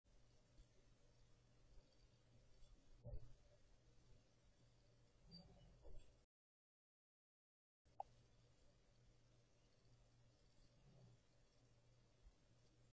MAGA Catrobatic Cat Azrael
Our Acrobatic Flying Feline Azrael, aka Azzy, is trying 2 get me back in our room by performing catrobatics.